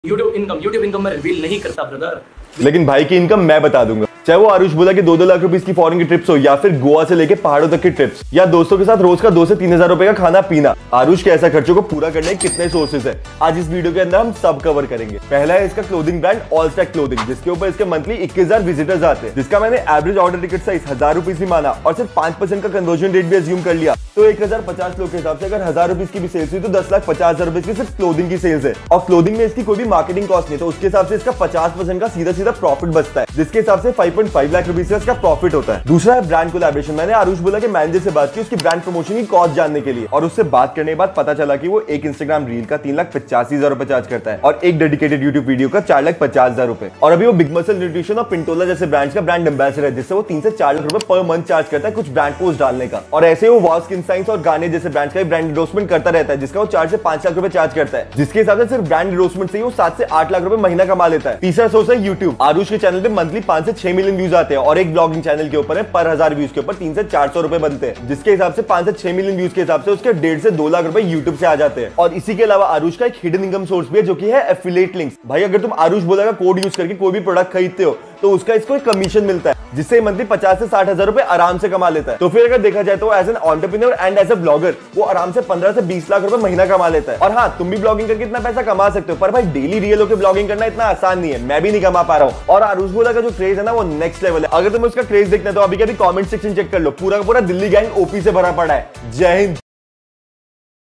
Aarush Bhola ke Hidden Income Sources 🤯✅
Aarush Bhola ke Income sources jaanke tumhare hosh udd jaenge 🤯

Basically ess video ke abdar aarush bhola kitna kamata hai aur kese mene sab cover kiya hai! ❤️

Video achi lage toh bell icon dava do mere doston 🔔